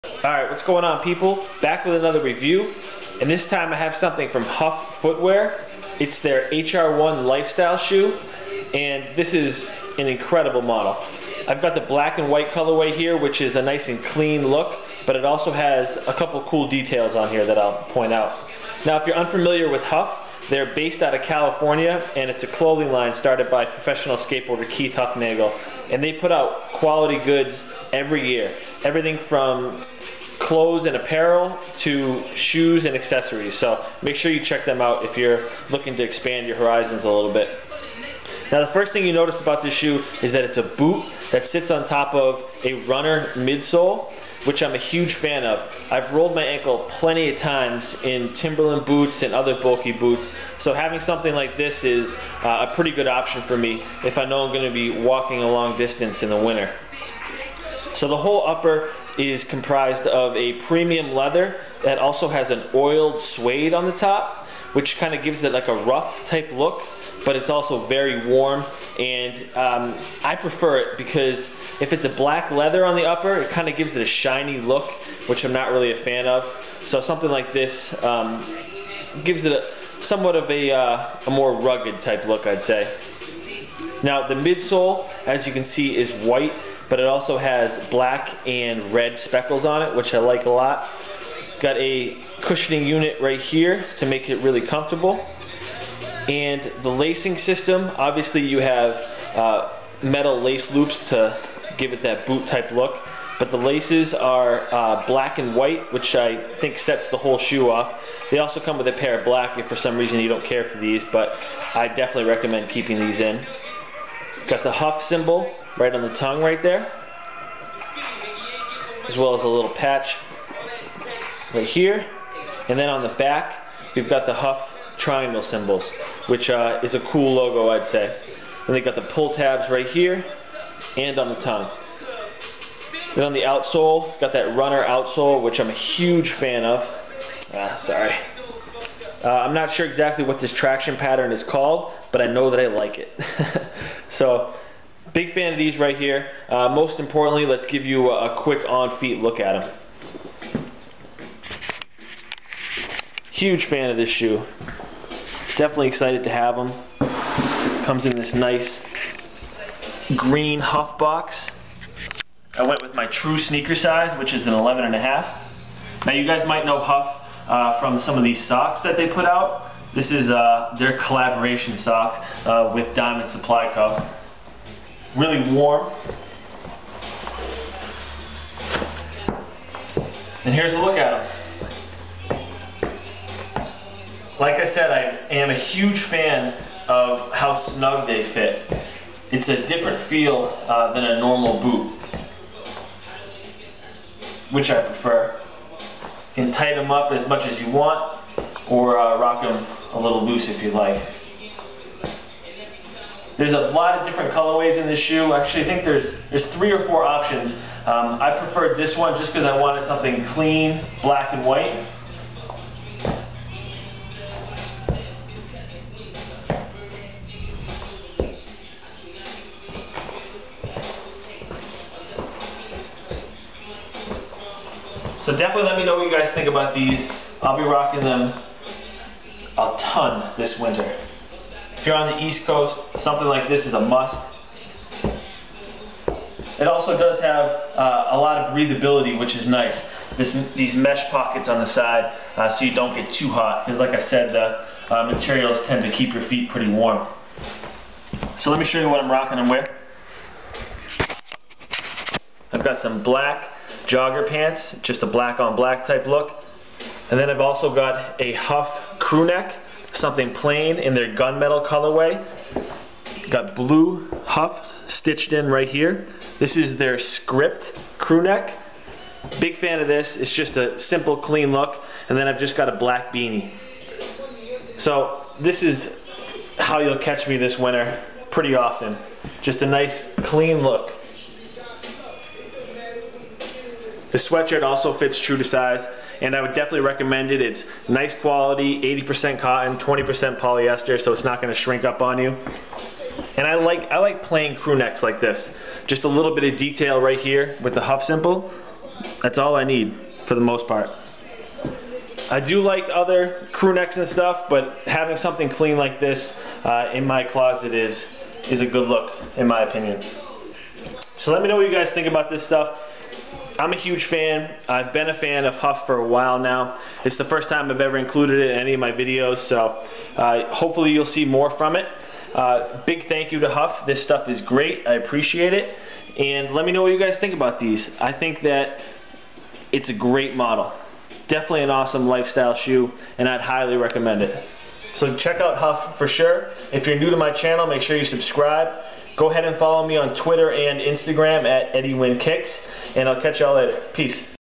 HUF Footwear HR-1 Lifestyle Boot ON FEET Review
Here's a quick look at a lifestyle boot from HUF. I've been rocking with them for a while and I'm stoked to finally get a review up on one of their models. And hit me up on Twitter and instagram @Eddiewinkicks. Peace!